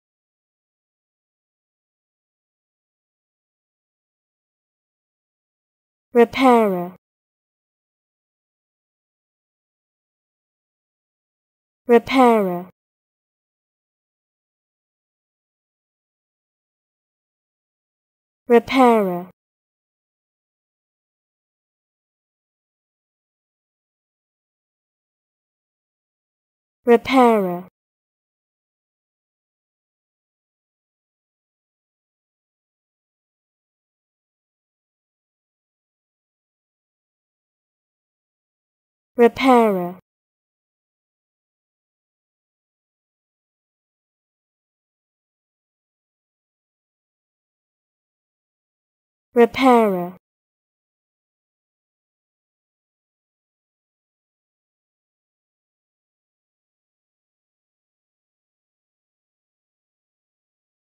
How To Say Repairer
Pronunciation of Repairer: Learn how to pronounce the word Repairer.